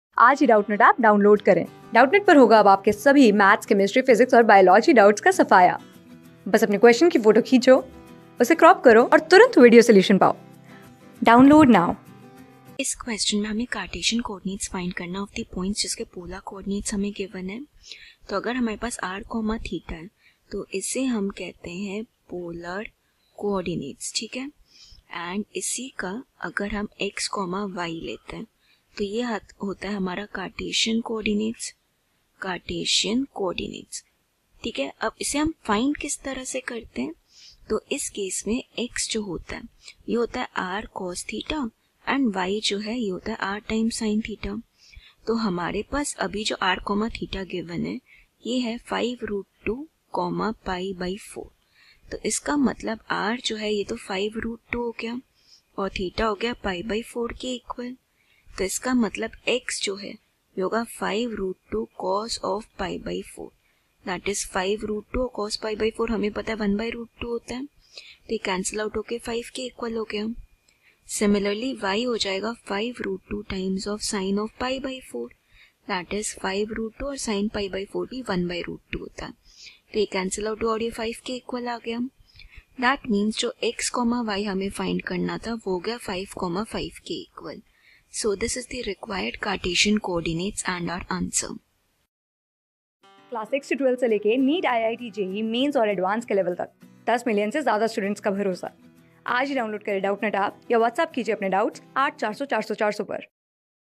Find the cartesian coordinates of the points whose polar coordinates are (5 sqrt(2), (pi)/(4))
Class: 12
Subject: MATHS
Chapter: COORDINATE SYSTEM AND COORDINATES
Board:IIT JEE

👉 Have Any Query? Ask Us.